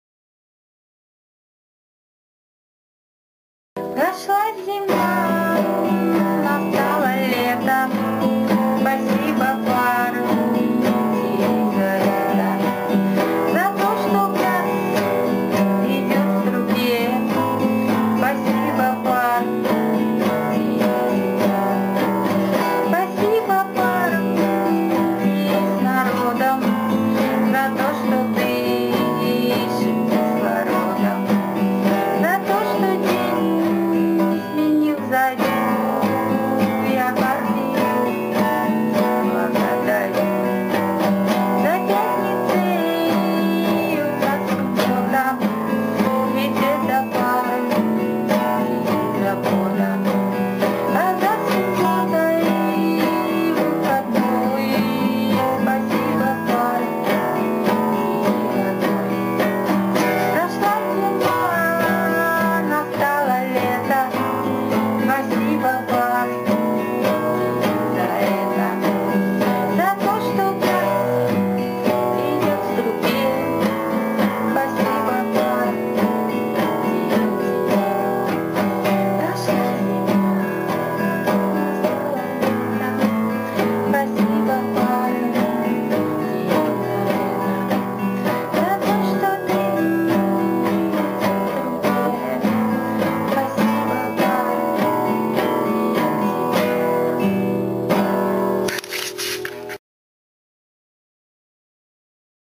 Юрий Влодов. Ода Партии.wmv
Этот клип сделан москвской поэтессой Людмилой Осокиной на известное диссидентское стих поэта Юрия Влодова "Ода Партии".
30 янв 2012 г.